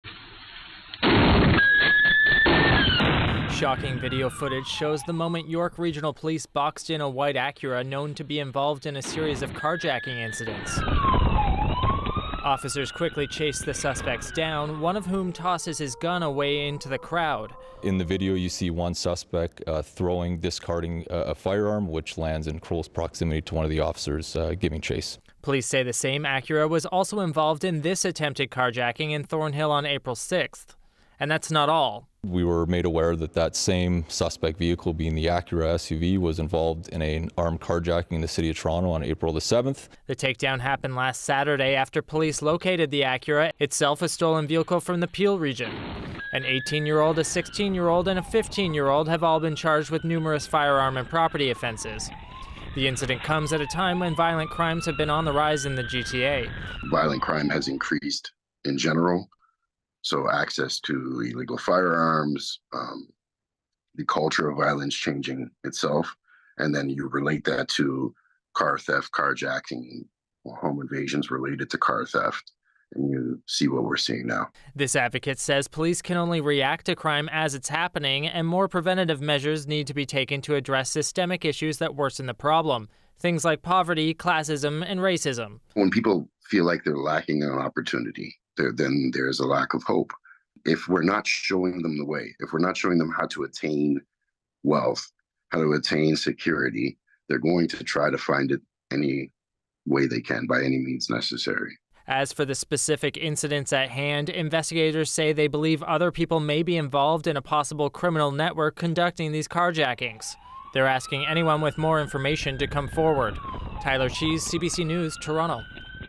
3 people charged following violent GTA carjacking
York Regional Police have released footage of a recent carjacking and takedown that occurred in Thornhill, Ont. This comes as the GTA has seen a significant rise in armed carjackings.